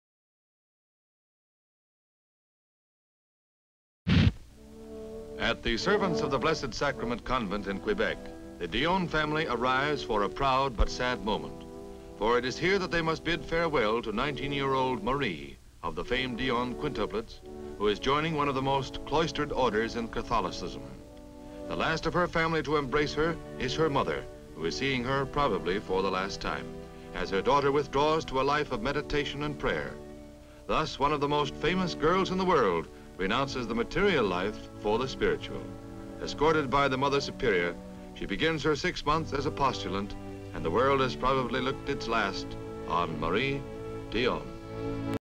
Marie Dionne, a quintuplet enters convent for the spiritual world in Canada. HD Stock Footage
Marie Dionne, a quintuplet enters convent for the spiritual world in Canada.

Family members and others bid farewell to Marie Dionne in Canada. She forsakes the material world for the spiritual. She is a quintuplet. People and her family members enter the Servants of the Blessed Sacrament Convent. Family members bid farewell to her. Location: Canada. Date: November 5, 1953.

57,000+ broadcast-quality historic clips for immediate download.